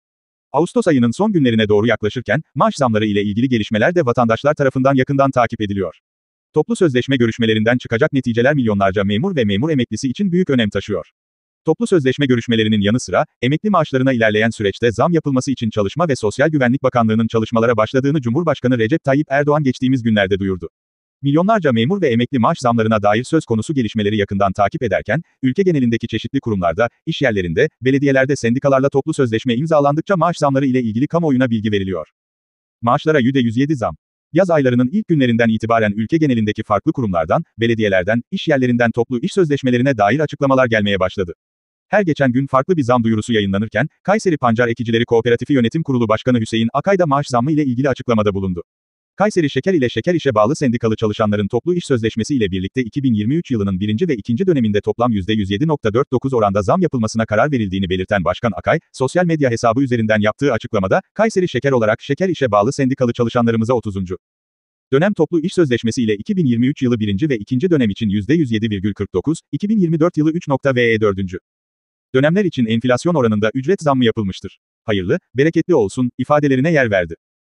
Rekor Maaş Zammı Müjdesi Geldi! Yüzde 107 Maaş Zammı Ağustos Ayında Açıklandı
Ağustos ayının son günlerine doğru yaklaşırken, maaş zamları ile ilgili gelişmeler de vatandaşlar tarafından yakından takip ediliyor.
Toplu sözleşme görüşmelerinden çıkacak neticeler milyonlarca memur ve memur emeklisi için büyük önem taşıyor. Toplu sözleşme görüşmelerinin yanı sıra, emekli maaşlarına ilerleyen süreçte zam yapılması için Çalışma ve Sosyal Güvenlik Bakanlığı'nın çalışmalara başladığını Cumhurbaşkanı Recep Tayyip Erdoğan geçtiğimiz günlerde duyurdu. Milyonlarca memur ve emekli maaş zamlarına dair söz konusu gelişmeleri yakından takip ederken, ülke genelindeki çeşitli kurumlarda, işyerlerinde, belediyelerde sendikalarla toplu sözleşme imzalandıkça maaş zamları ile ilgili kamuoyuna bilgi veriliyor

Maaşlara Yüde 107 Zam
Yaz aylarının ilk günlerinden itibaren ülke genelindeki farklı kurumlardan, belediyelerden, iş yerlerinden toplu iş sözleşmelerine dair açıklamalar gelmeye başladı. Her geçen gün farklı bir zam duyurusu yayınlanırken, Kayseri Pancar Ekicileri Kooperatifi Yönetim Kurulu Başkanı Hüseyin Akay da maaş zammı ile ilgili açıklamada bulundu. Kayseri Şeker ile Şeker İş'e bağlı sendikalı çalışanların toplu iş sözleşmesi ile birlikte 2023 yılının birinci ve ikinci döneminde toplam yüzde 100 7.49 oranda zam yapılmasına karar verildiğini belirten Başkan Akay, sosyal medya hesabı üzerinden yaptığı açıklamada ‘’Kayseri Şeker olarak Şeker-İş’e bağlı sendikalı çalışanlarımıza 30. Dönem toplu iş sözleşmesi ile 2023 yılı birinci ve ikinci dönem için yüzde 107.49, 2024 yılı 3.ve 4. Dönemler için enflasyon oranında ücret zammı yapılmıştır. Hayırlı, bereketli olsun’’ ifadelerine yer verdi. #2023